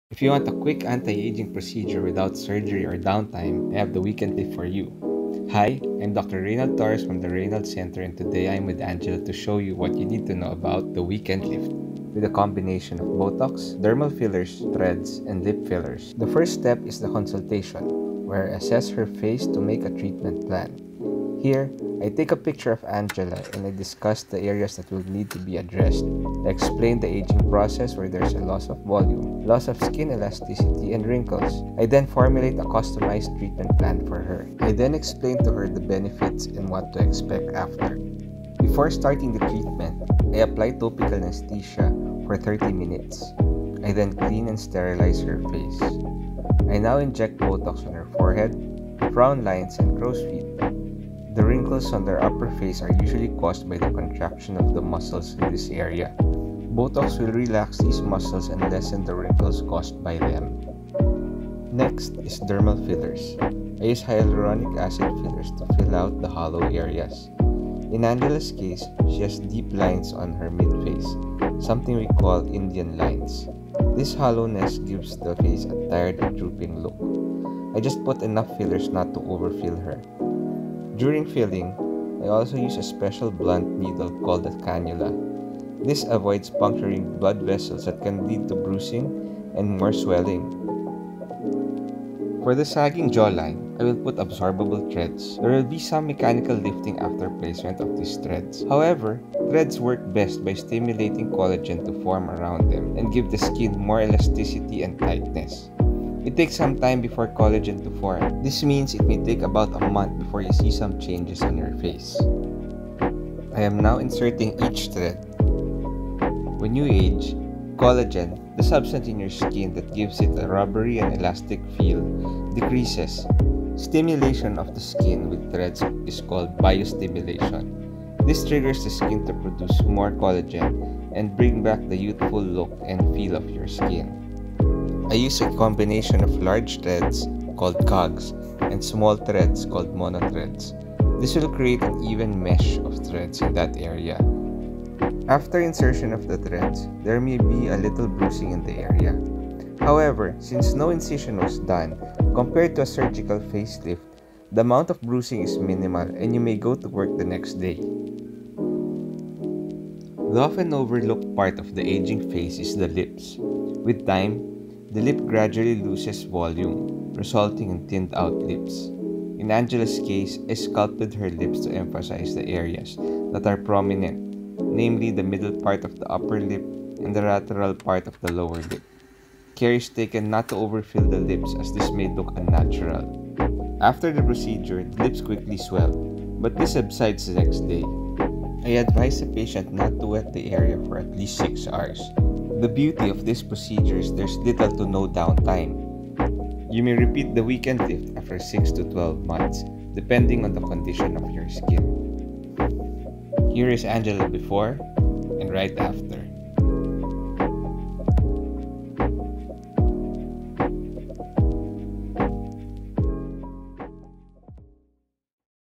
How to Look 10 Years Younger Over the Weekend with No Surgery
Need a lift but too young for face lift? Here's how to look 10 years younger over the weekend with The Weekend Lift at The Raynald Center by Dr. Raynald Torres.

Part 1: Consultation and Assessment
During the consultation, Dr. Raynald explains the natural process of skin aging to the patient and assesses their current condition. Based on this assessment, he provides a personalized treatment recommendation.

Part 2: Botox
Dr. Raynald uses Botox injections to address fine lines and wrinkles on the upper face, including the forehead lines, frown lines, and crow's feet.

Part 3: Dermal Fillers
As a common sign of aging, loss of volume on the face can lead to a tired, haggard appearance. Dr. Raynald addresses this by injecting fillers into areas of the face that need volume, providing a plumper, more youthful appearance.

Part 4: Thread Lift
To counteract sagging skin, Dr. Raynald uses dissolvable threads to mechanically lift the skin and promote the natural production of collagen. This helps enhance skin elasticity, resulting in firmer, tighter skin.

Part 5: Lip Fillers
As we age, our lips tend to thin and lose volume. Dr. Raynald addresses this with lip filler injections that volumize and hydrate the lips for a younger, fuller appearance.

📍Alabang - Mon to Sun • 9am to 8pm • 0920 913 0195 | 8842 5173
📍BGC - Mon to Sat • 10am to 6pm • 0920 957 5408 | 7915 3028